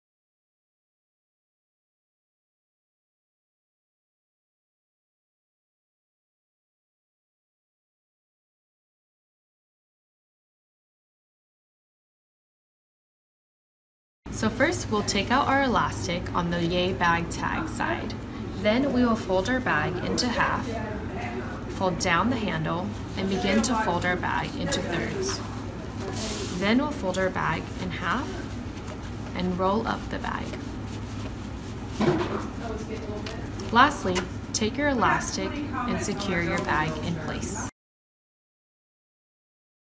YaY Novelty How to Fold YaYbag
How to fold YaYbag by YaY Novelty.

YaYbag is water resistant and machine washable. It folds into a small roll and secure with a built-in elastic band for compact and easy storage.

YaYbag is made of strong 100% polyester and can hold up to 55 lbs. Superior craftsmanship using Nylon thread and double stitching. Clean finish interior seam with binding.

Dimension:
YaYbag ORIGINAL (small with short handles): 16"W x 20"H - Weight: 1.6oz
YaYbag JUMBO (large with long handles): 20"W x 28"H - Weight: 2oz